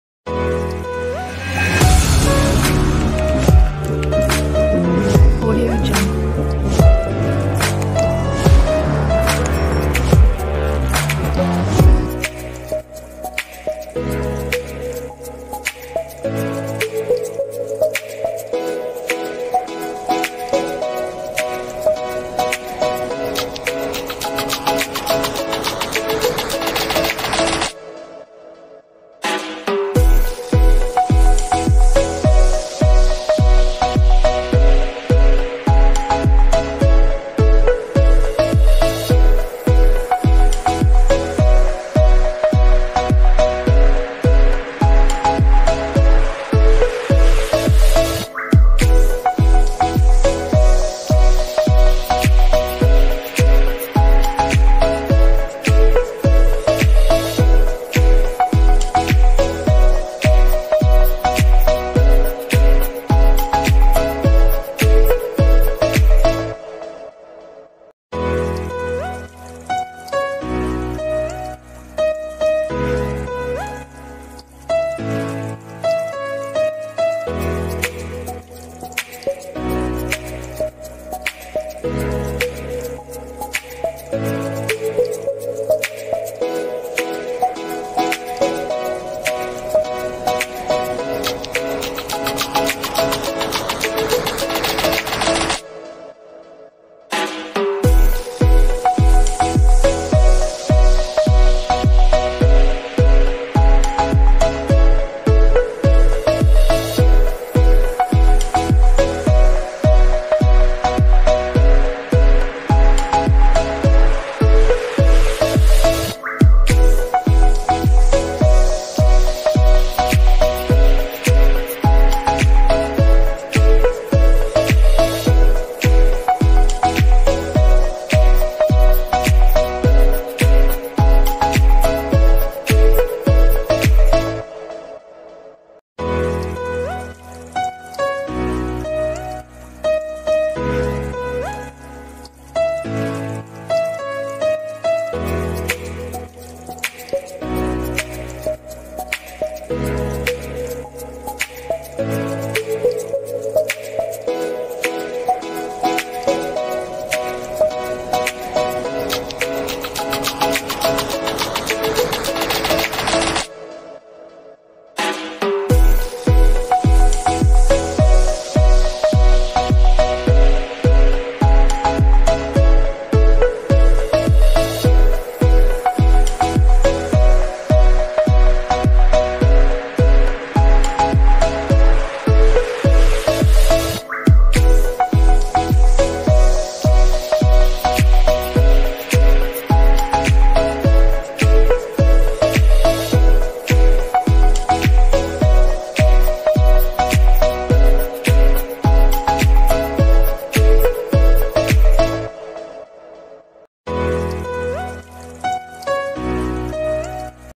DIY wooden lantain ll wooden stick ll home decor II magic craft for crafties
Hey guys!
Today we will be making a beautiful lantain at home with ice cream sticks and wooden sticks.